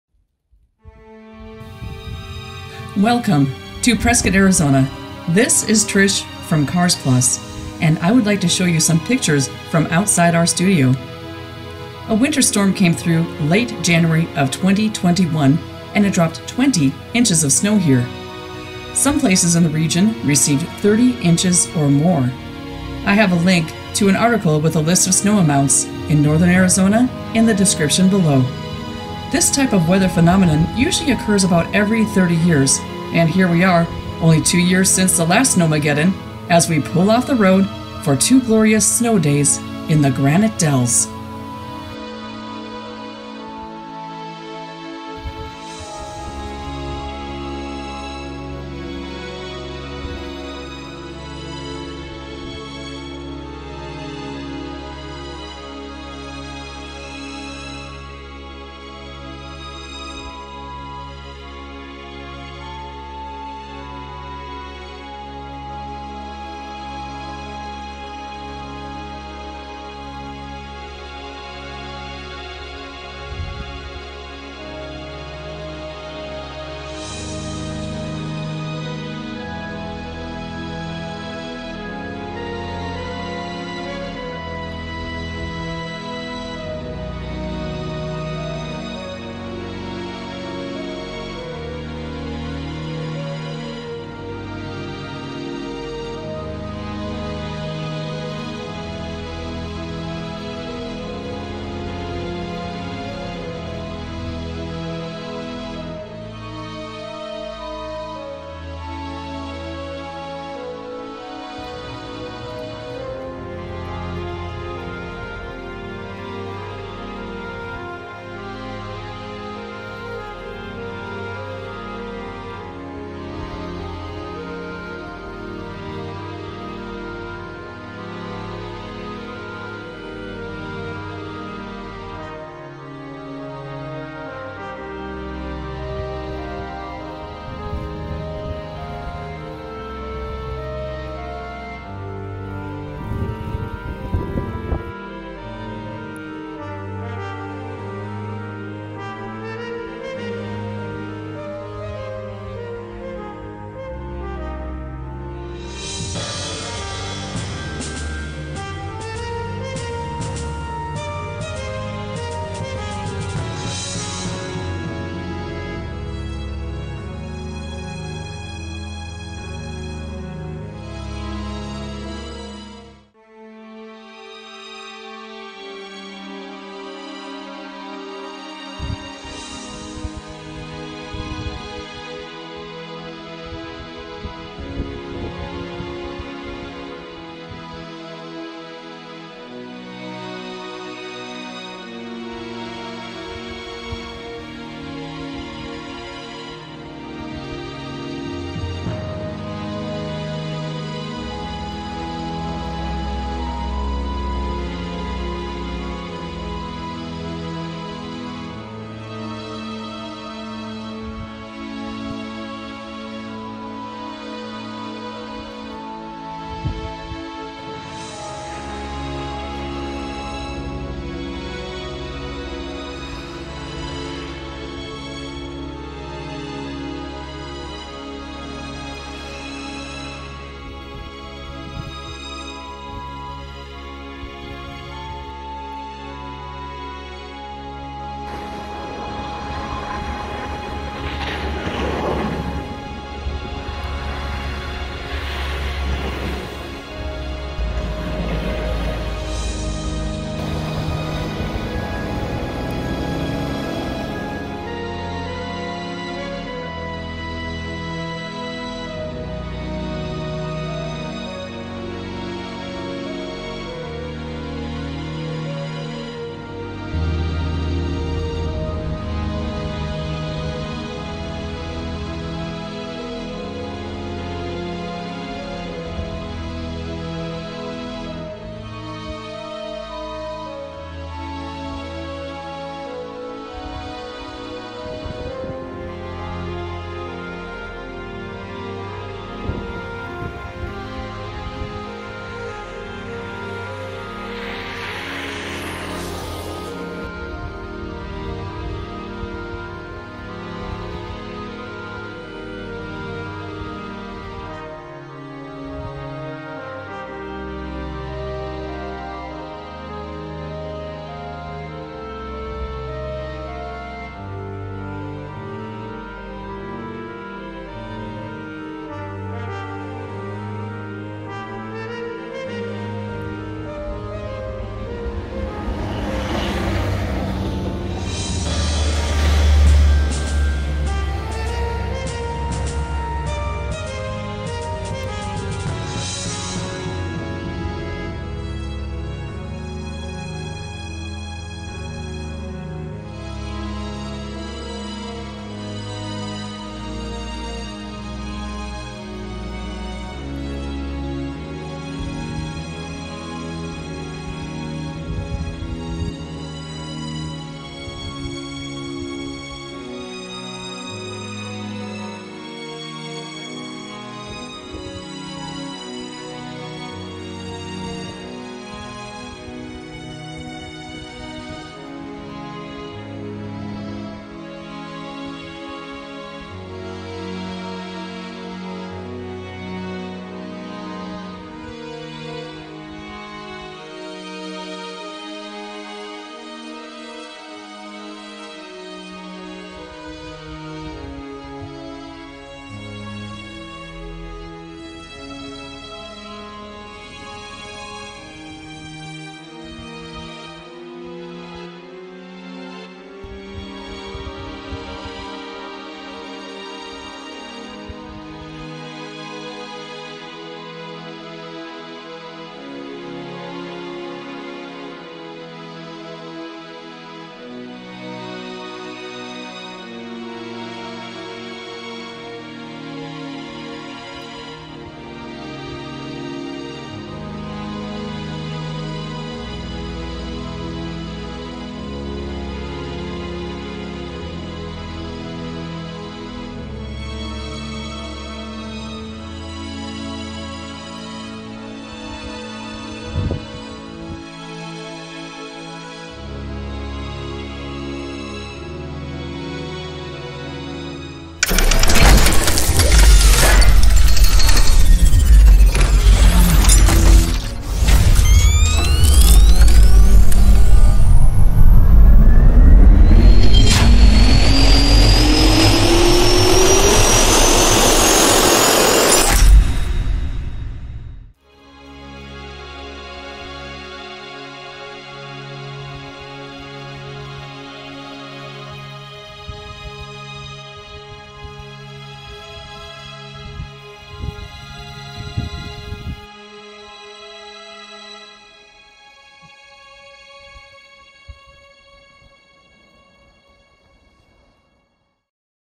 Snow Storm Rare Weather Event in Prescott Arizona - Snowmageddon January 2021 Northern Arizona
Welcome to Prescott Arizona where the weather will sometimes become a snow filled event just like it did in this year's Snowmageddon. A winter storm came through late January of 2021 and it droppped 20 inches of snow. Some places in the region received 30 inches of snow or more. This type of weather phenomenon usually occurs about every 30 years, and here we we are only 2 years since the last  snowmageddon in Prescott, February 21 - 22, 2019. I'd like to show you some pictures from outside our studio, so pull off the road with us and see the highlights from two glorious snow days in the Granite Dells.

Here is a link for you to check out Northern Arizona's Snowfall

This snow storm even attracted a news crew, Channel 3 & CBS 5, from Phoenix. Here's a contribution from AZ Family

The music in this video is courtesy of YouTube sound collection.